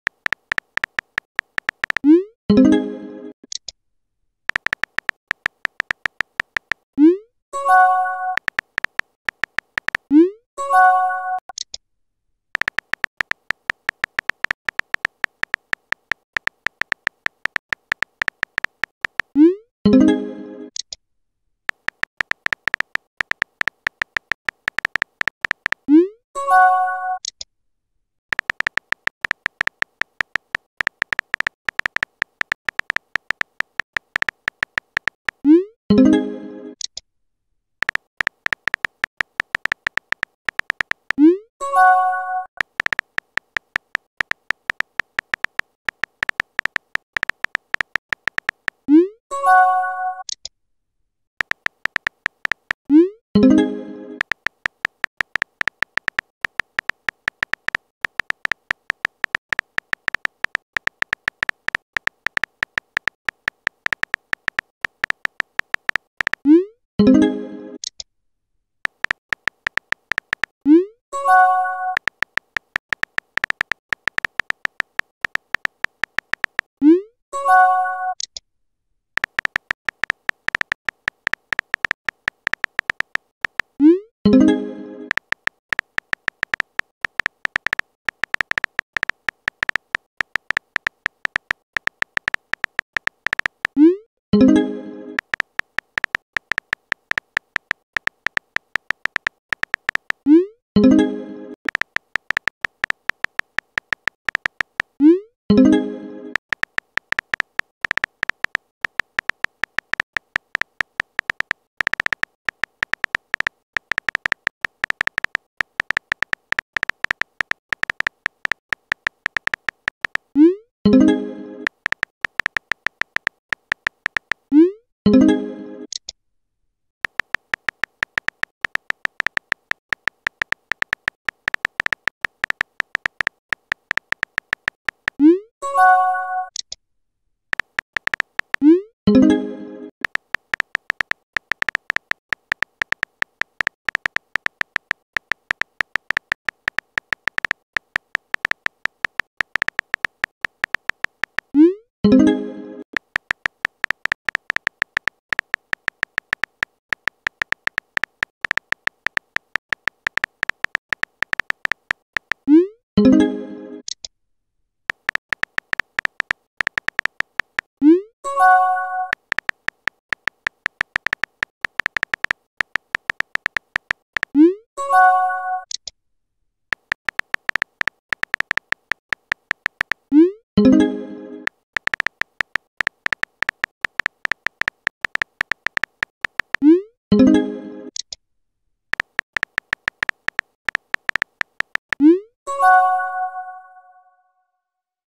[Mini Text Story] Wonhyuk ft. Kihyun (Part 1)
There will be a 2nd part to this (I'll write it in the morning)

I tried to add Kihyun to the thumbnail but there wasn't enough room TT